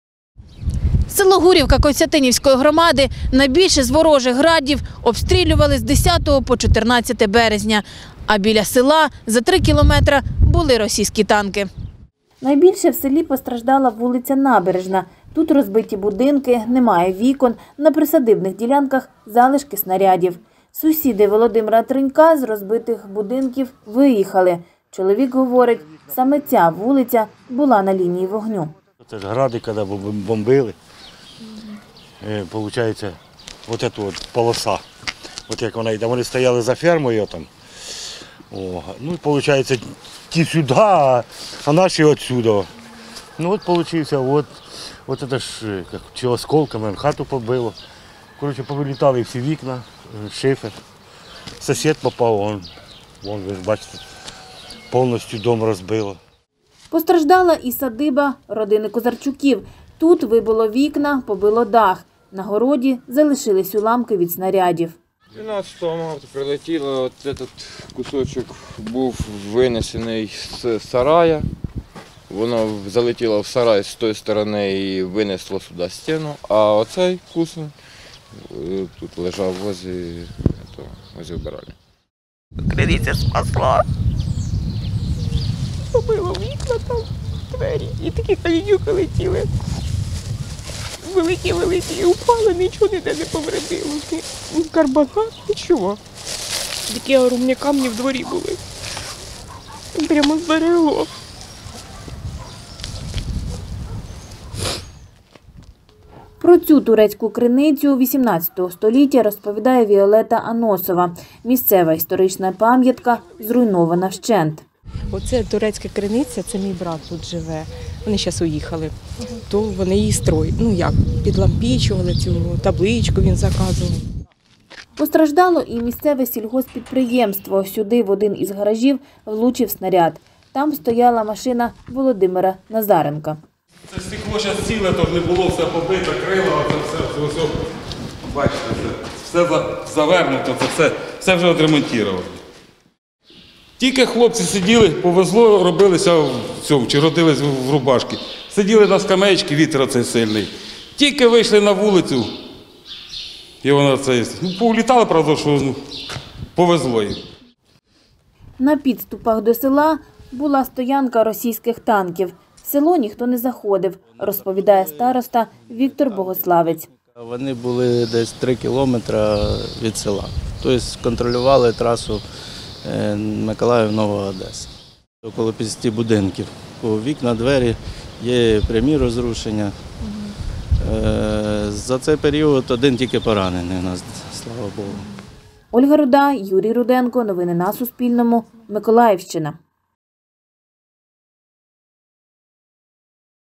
Наслідки обстрілу села Гур'ївка. Що розповідають люди
Розбиті будинки, уламки снарядів на присадибних ділянках та зруйнована історична пам'ятка. Це побачили кореспонденти Суспільного у селі Гур'ївка Костянтинівської громади.
Авторка: Ольга Руда.